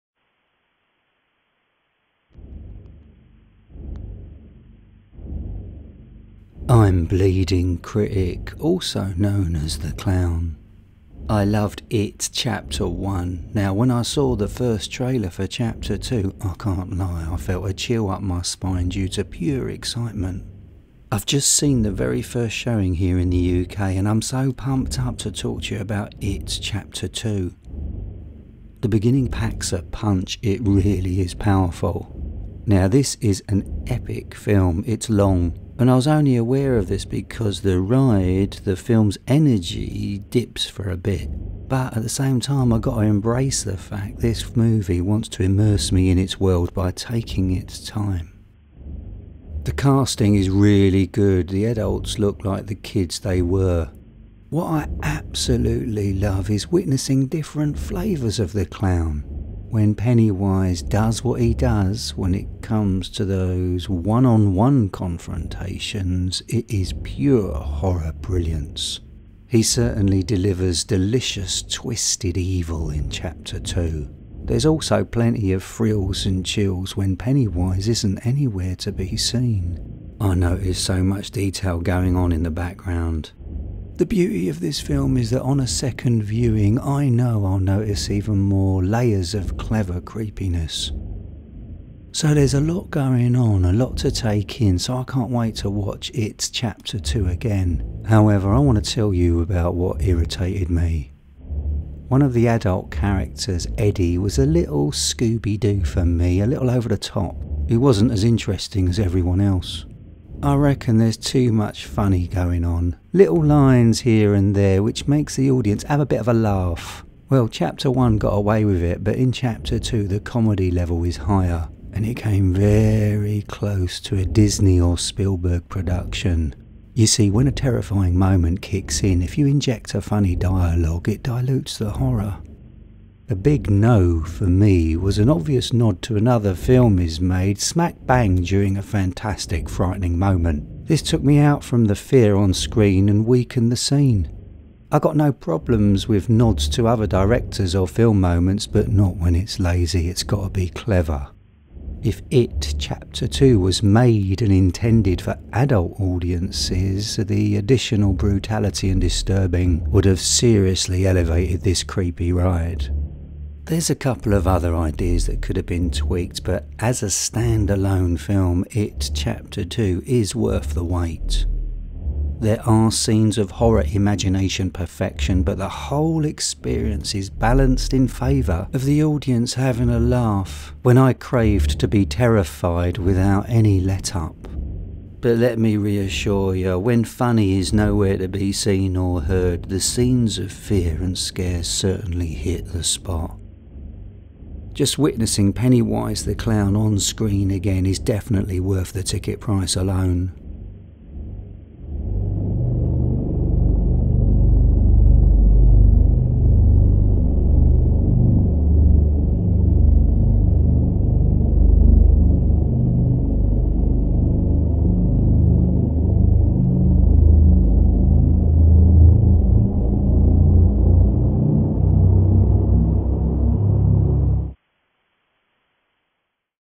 IT CHAPTER TWO review with BLEEDING CRITIC
I’ve just seen the very first showing here in the UK, and I’m so excited to talk to you about IT CHAPTER TWO.
IT CHAPTER TWO review written and narrated by BLEEDING CRITIC